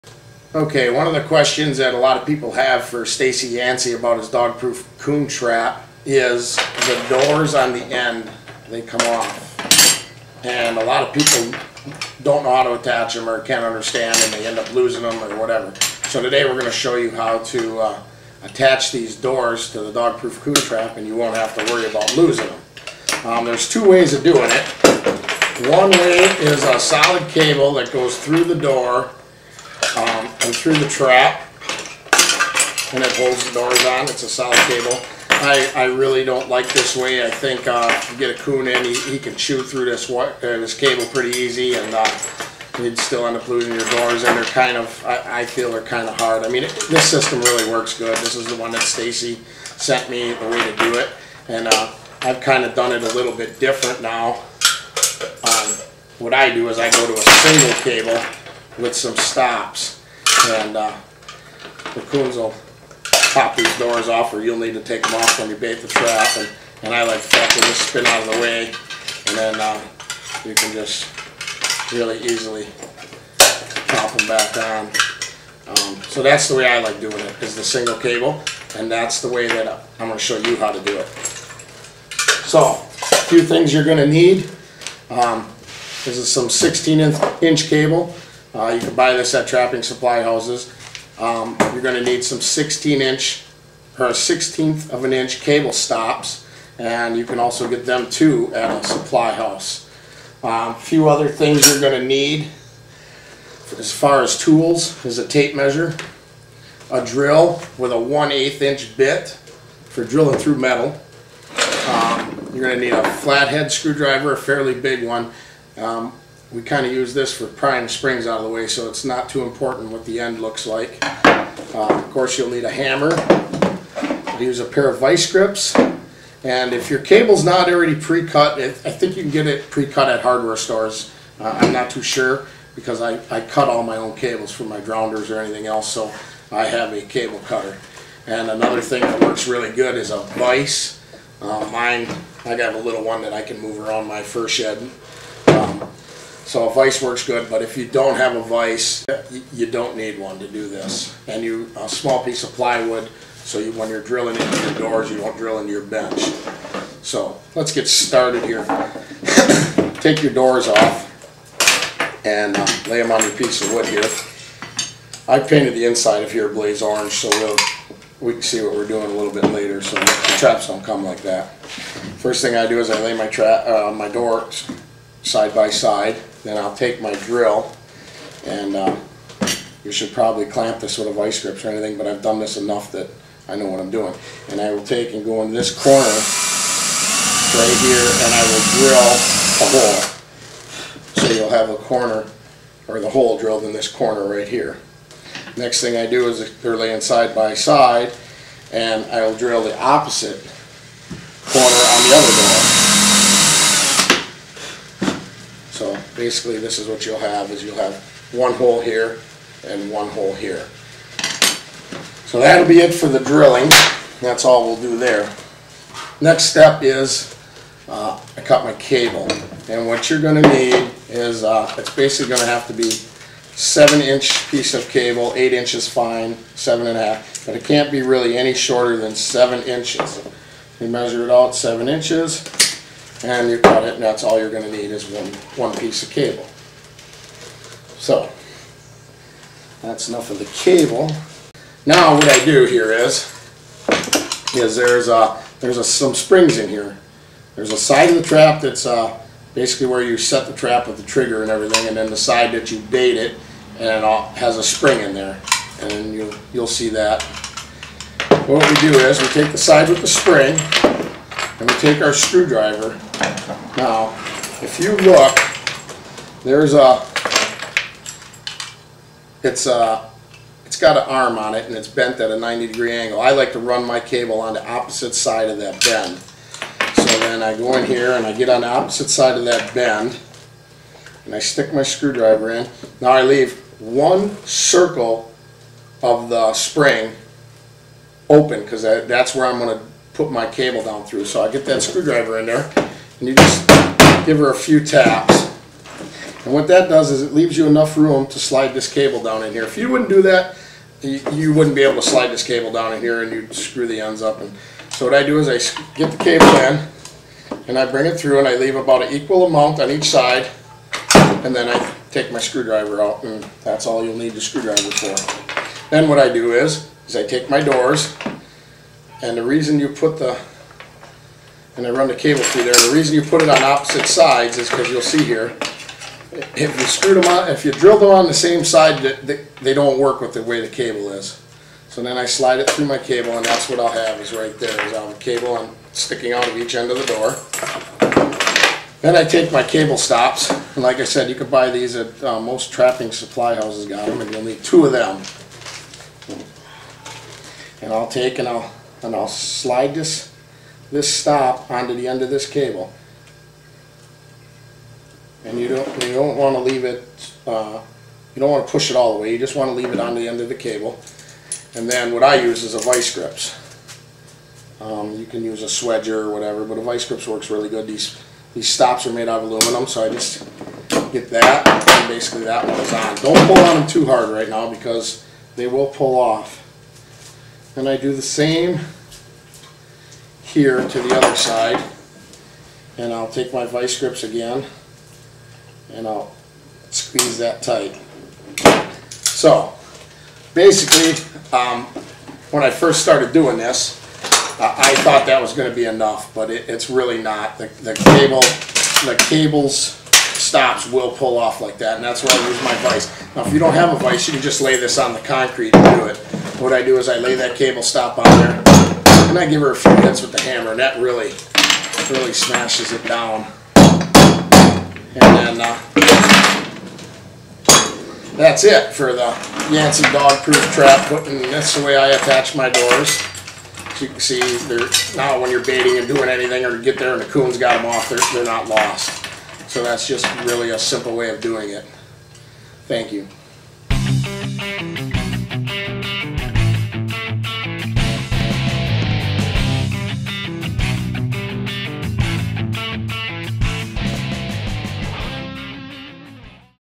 yancy door modification
how i attach the doors to the yancy dogproof trap
Instagram and Twitter samwoodoutdoors